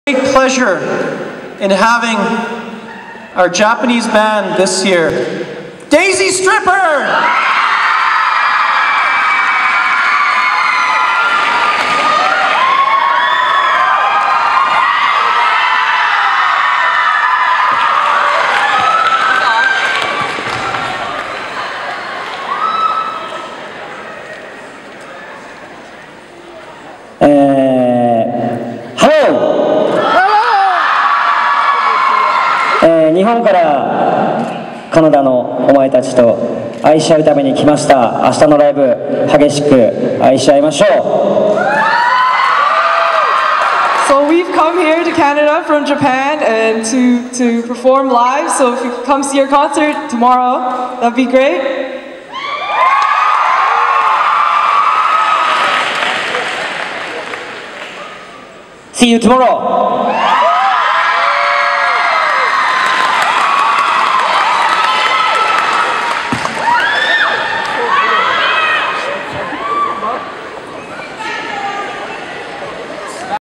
DaizyStripper Animethon Open Ceremonies
DaizyStripper at Opening Ceremonies Animethon 19 Edmonton Alberta